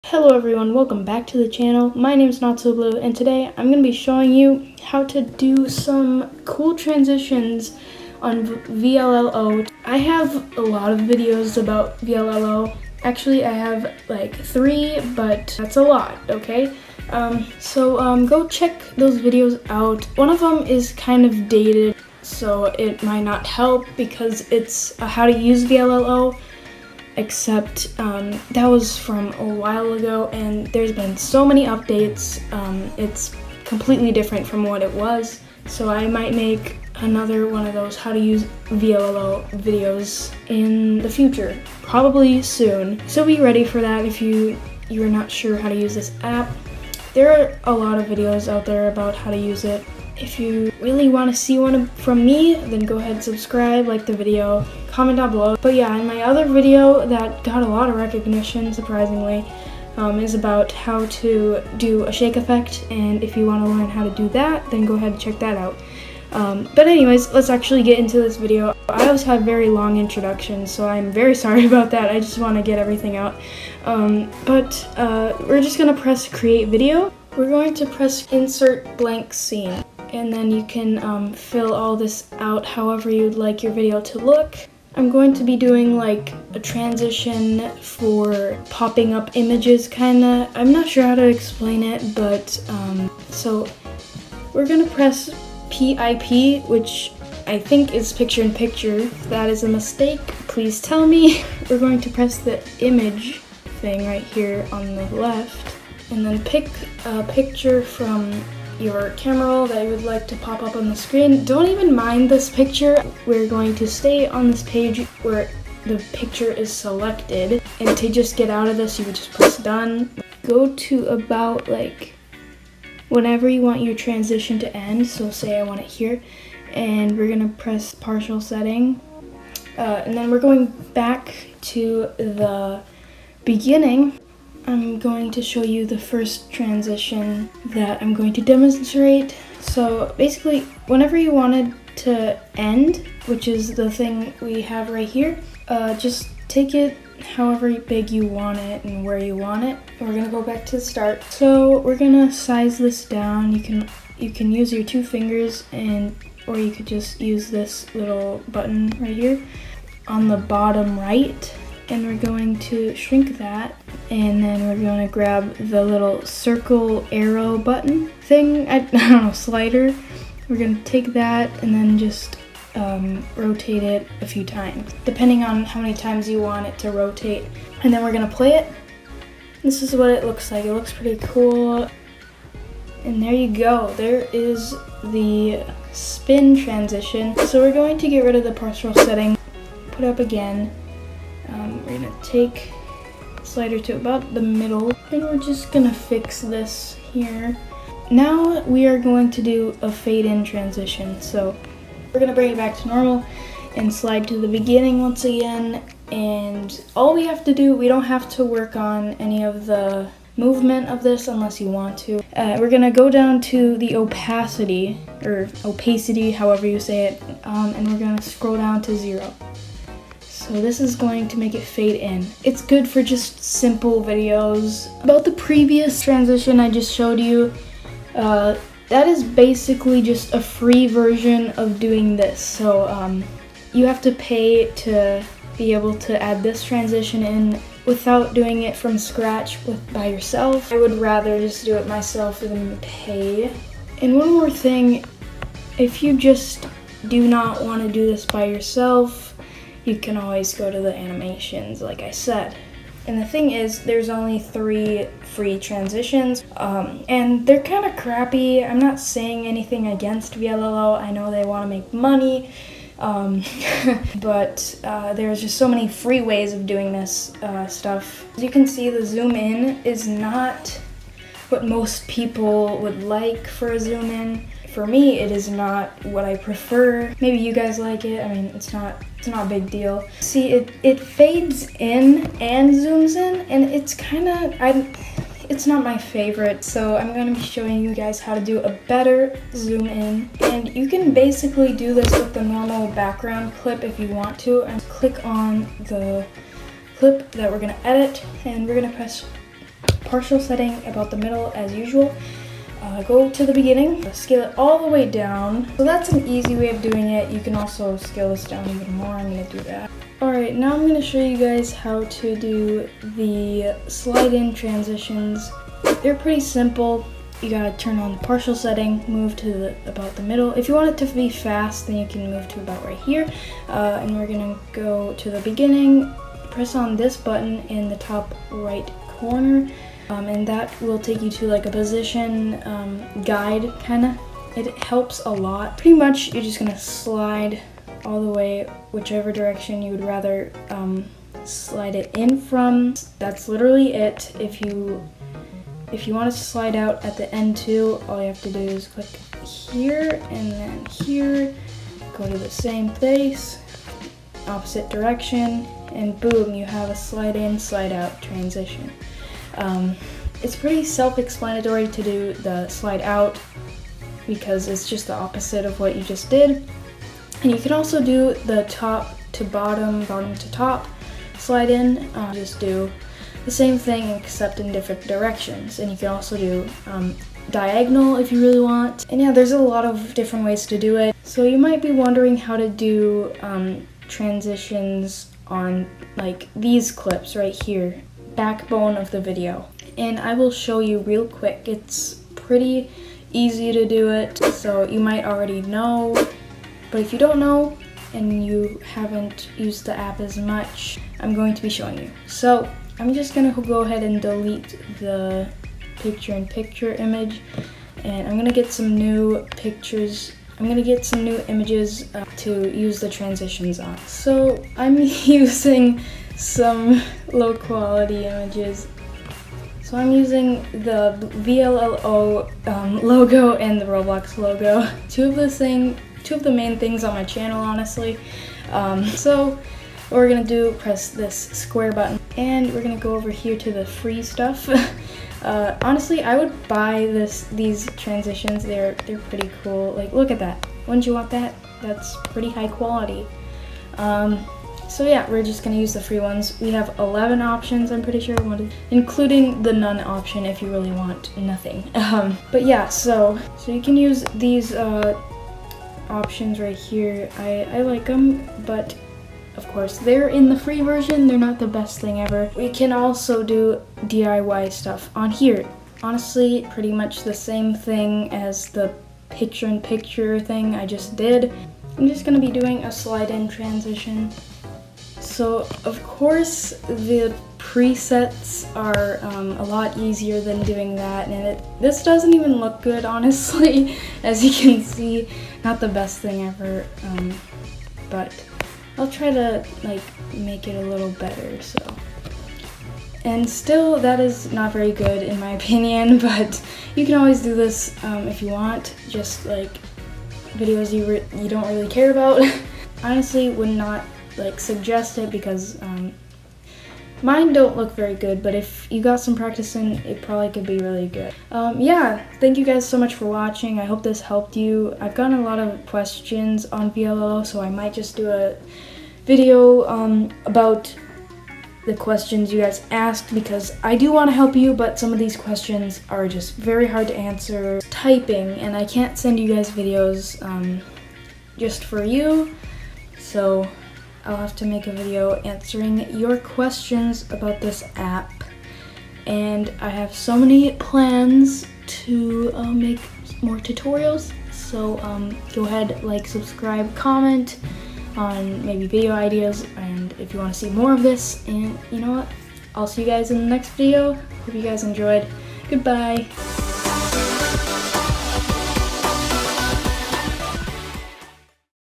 Free Transitions on VLLO! | VLLO Tutorials
Hey BluEberries!ヽ(͡◕ ͜ʖ ͡◕)ﾉ In this video I will show you guys how to do some cool transitions in the free app VLLO.